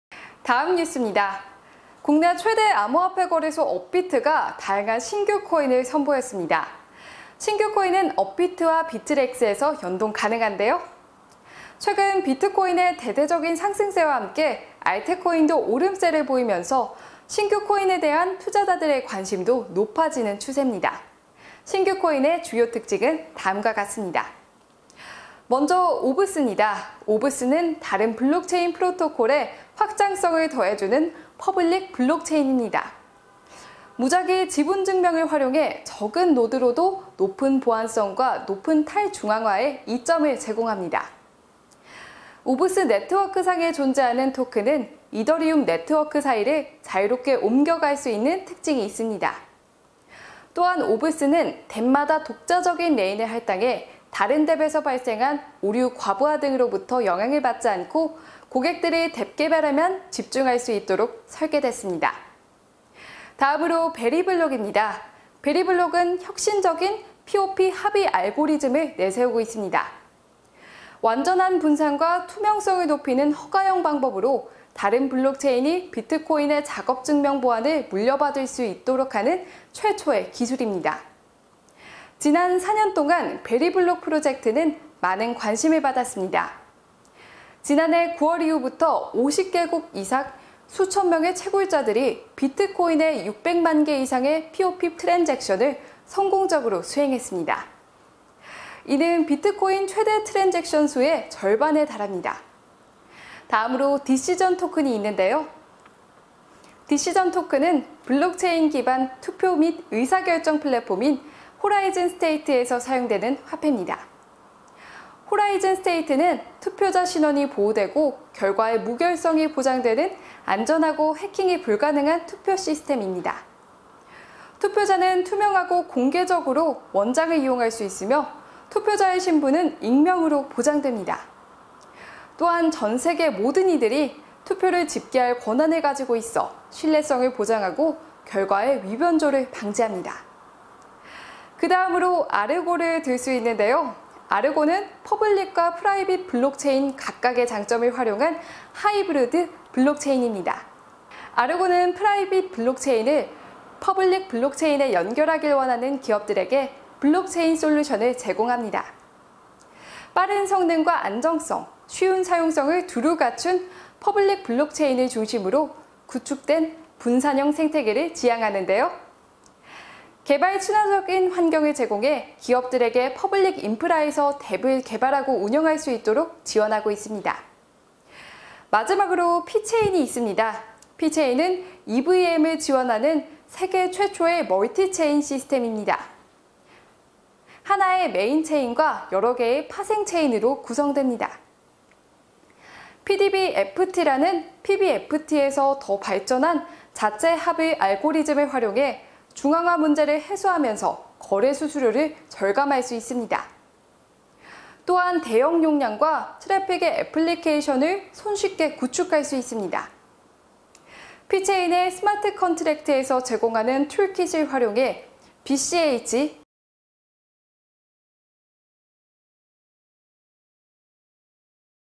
[라이브썰] 업비트, ‘아르고’ 등 신규 코인 입성 … 불마켓 투자자 관심 증대
국내 최대 암호화폐 거래소 업비트가 다양한 신규 코인을 선보였습니다. 신규 코인은 업비트와 비트렉스에서 연동 가능합니다.

최근 비트코인의 대대적인 상승세와 함께 알트코인도 오름세를 보이면서 신규 코인에 대한 투자자들의 관심도 높아지는 추세입니다. 신규 코인 주요 특징은 다음과 같습니다.

BTC마켓: Orbs(오브스 ORBS)

오브스는 다른 블록체인 프로토콜에 확장성을 더해주는 퍼블릭 블록체인이다. 무작위 지분증명(rPoS)을 활용해 적은 노드로도 높은 보안성과 높은 탈중앙화의 이점을 제공한다. 오브스 네트워크 상에 존재하는 토큰은 이더리움 네트워크 사이를 자유롭게 옮겨갈 수 있는 특징이 있다. 또한 오브스는 DApp마다 독자적인 레인을 할당해 다른 DApp에서 발생한 오류, 과부하 등으로부터 영향을 받지 않고 고객들이 DApp 개발에만 집중할 수 있도록 설계됐다.

BTC마켓: VeriBlock(베리블록 VBK)

베리블록은 혁신적인 PoP(Proof-of-Proof) 합의 알고리즘을 내세우고 있다. 완전한 분산과 투명성을 높이는 비허가형 방법으로 다른 블록체인이 비트코인의 작업증명(PoW) 보안을 물려받을 수 있도록 하는 최초의 기술이다. 지난 4년 동안 베리블록 프로젝트는 많은 관심을 받았다. 지난해 9월 이후부터 50개국 이상 수천 명의 채굴자들이 비트코인에 600만 개 이상의 PoP 트랜잭션을 성공적으로 수행했다. 이는 비트코인 최대 트랜잭션 수에 절반에 달한다.

BTC마켓: Decision Token(디시전토큰 HST)

디시전토큰은 블록체인 기반 투표 및 의사결정 플랫폼인 Horizon State에서 사용되는 화폐다. Horizon State는 투표자 신원이 보호되고 결과의 무결성이 보장되는 안전하고 해킹이 불가능한 투표 시스템이다. 투표자는 투명하고 공개적으로 원장을 이용할 수 있으며 투표자의 신분은 익명으로 보장된다. 또한 전 세계 모든 이들이 투표를 집계 권한을 가지고 있어 신뢰성을 보장하고 결과의 위변조를 방지한다.

BTC마켓: Aergo(아르고 AERGO)

아르고는 퍼블릭과 프라이빗 블록체인 각각의 장점을 활용한 하이브리드 블록체인이다. 아르고는 프라이빗 블록체인을 퍼블릭 블록체인에 연결하길 원하는 기업들에게 블록체인 솔루션을 제공한다. 빠른 성능과 안전성, 쉬운 사용성을 두루 갖춘 퍼블릭 블록체인을 중심으로 구축된 분산형 생태계를 지향한다. 개발 친화적인 환경을 제공해 기업들에게 퍼블릭 인프라에서 DApp을 개발하고 운영할 수 있도록 지원하고 있다.

BTC마켓: PCHAIN(피체인 PI)

피체인은 EVM(Ethereum Virtual Machine)을 지원하는 세계 최초의 멀티 체인 시스템이다. 하나의 메인 체인과 여러 개의 파생 체인으로 구성된다. PDBFT라는 PBFT에서 더 발전한 자체 합의 알고리즘을 활용해 중앙화 문제를 해소하면서 거래 수수료를 절감할 수 있다. 또한 대형 용량과 트래픽의 애플리케이션을 손쉽게 구축할 수 있다. 피체인의 스마트컨트랙트에서 제공하는 툴킷을 활용해 BCH, ERC20 등 Non-native 체인에 쉽게 적용할 수 있다.